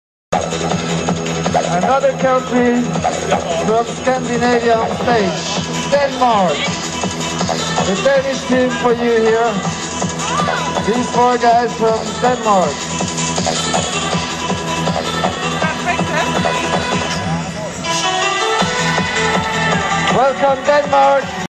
Danish Demo at Interski in Crans Montana, Switzerland 2003
Danish Demo (only 4 Danish delagations) at Interski in Crans Montana 2003.  1=Klavs Klavsen   2=Rasmus Lundby  3=Thomas Roennau  4=Lasse Lyck